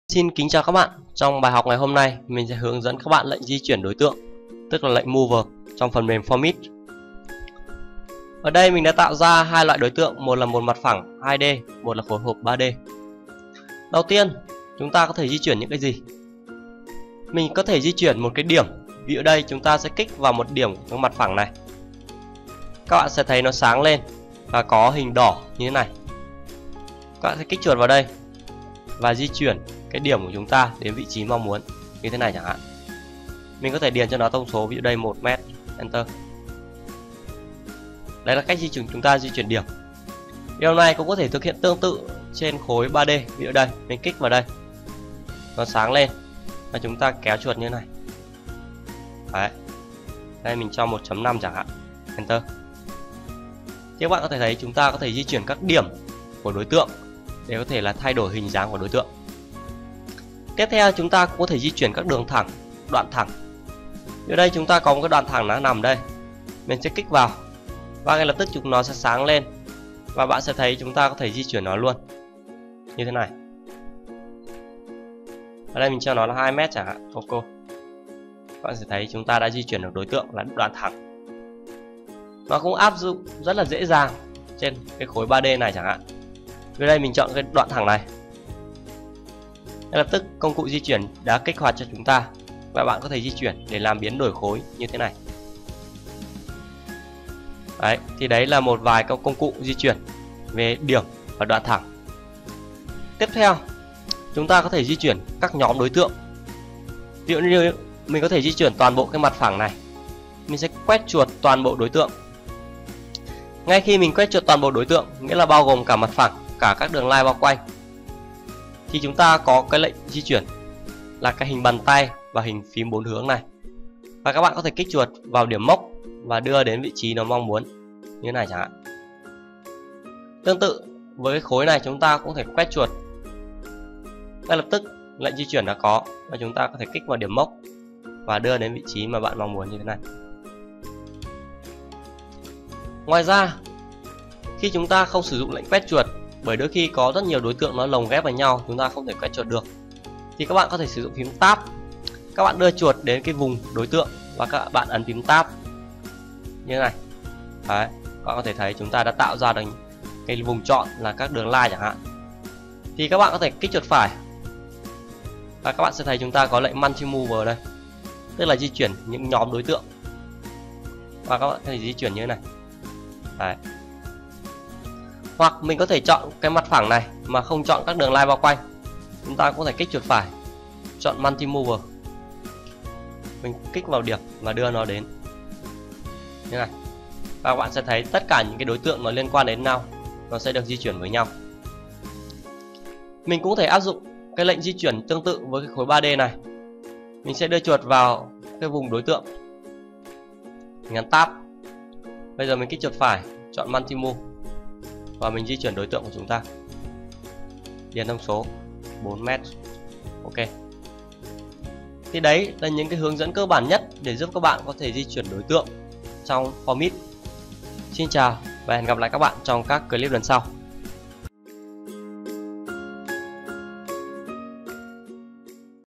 [HỌC FORMIT360 FREE] - Bài 13: Di chuyển đối tượng trong Formit
Formit 360 là phần mềm thiết kế concept kiến trúc rất mới của Autodesk. Rất mạnh mẽ và thú vị mà ít người biết đến.
Bộ video do chính tôi (hungphamkts2014) thực hiện trước đây, nay chia sẻ free cho các bạn yêu thích.